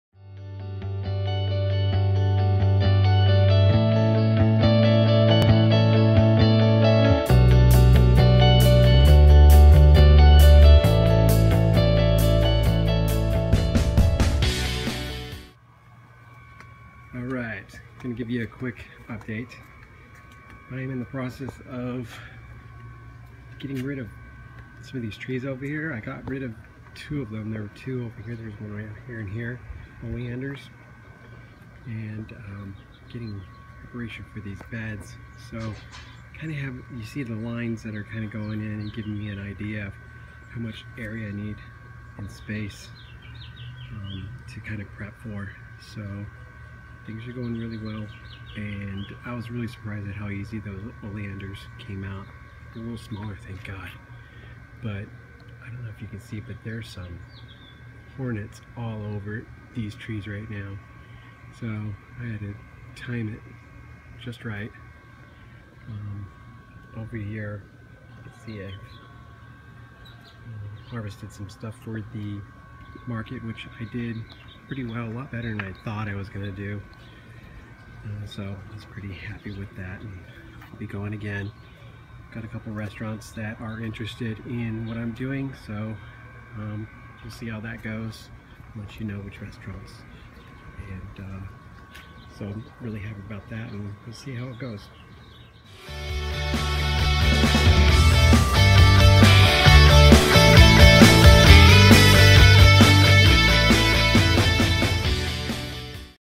Took some oleander out. Hornets!
Took oleander trees out to make more room for three more beds. Hornets are not happy with me.